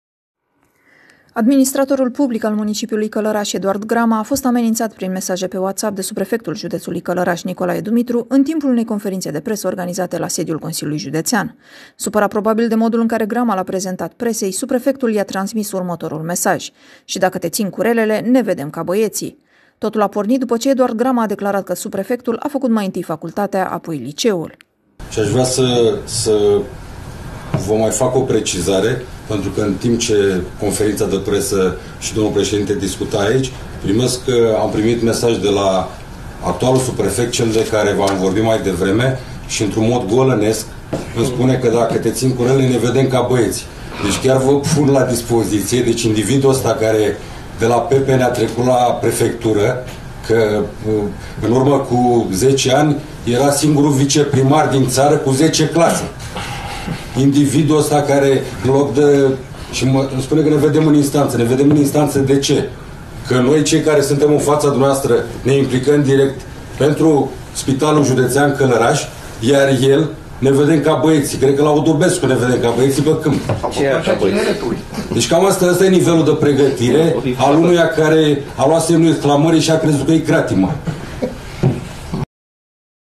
Amenințări, în direct, în timpul unei conferințe de presă
Administratorul public al municipiului Călărași, Eduard Grama a fost amenințat prin mesaje pe WhatsApp, de subprefectul județului Călărași, Nicolae Dumitru, în timpul unei conferințe de presă organizate la sediul Consiliului Județean.